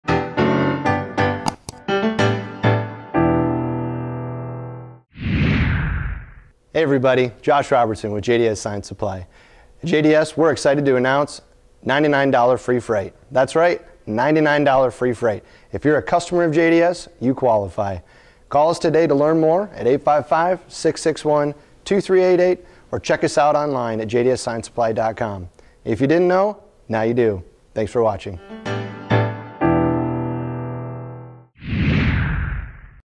JDS Insider #32 - $99 Free Freight for Sign Supply
JDS Sign Supply customers are now eligible for free freight on orders of $99 or more. Some restrictions do apply.